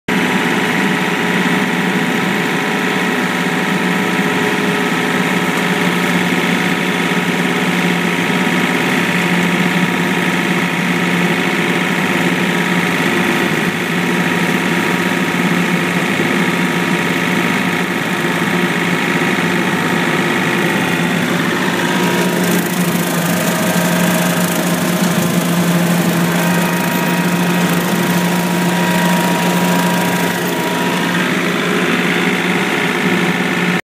New John Deere tractor and Bush Hog
Trying out the new tractor in a pasture of blue stem grass that has been overgrown for a lonnnnng time.